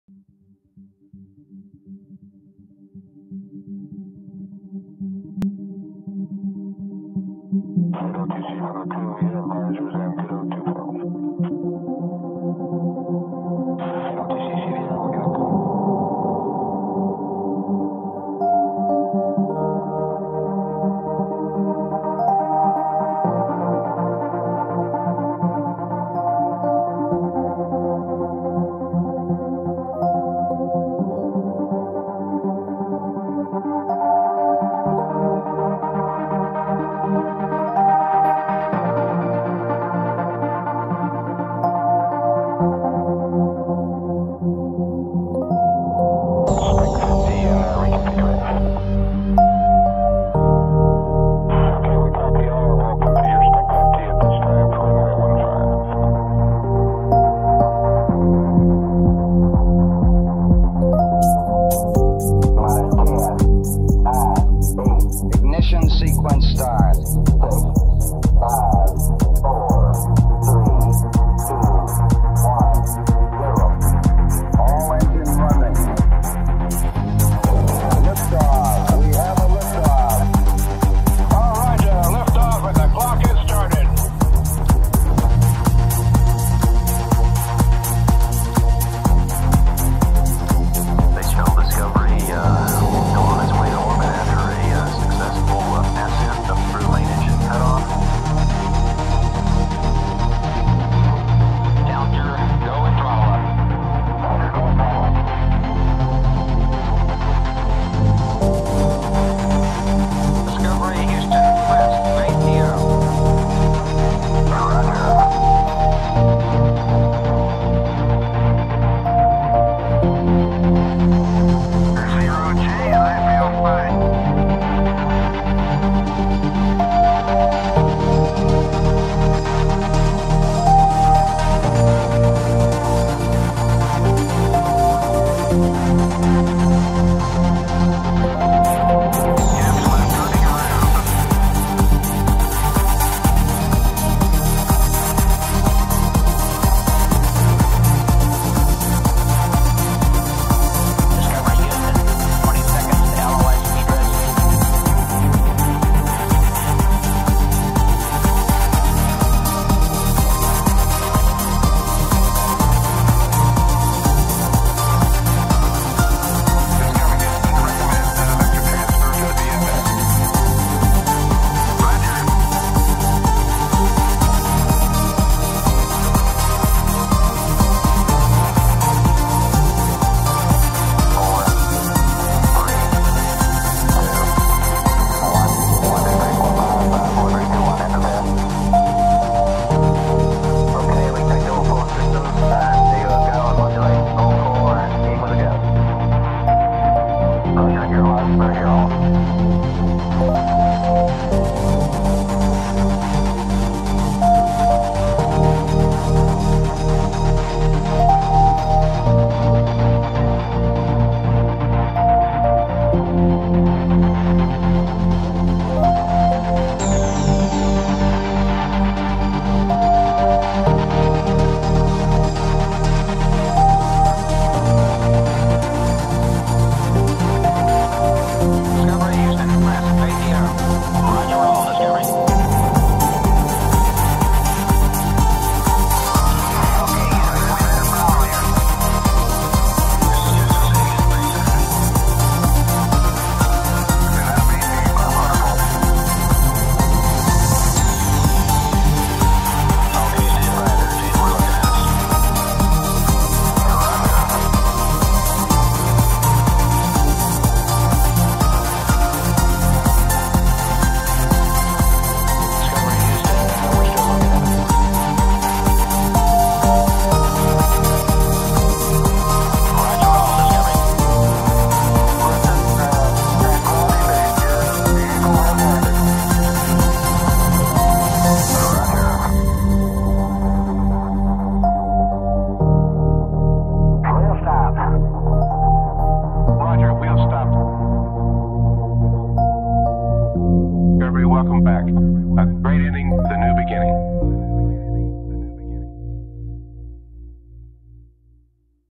Airstair Vincent Liz Colleen - Skyland 2022 - In Multi-Dimensional Surround Sound
In Multi-Dimensional Surround Sound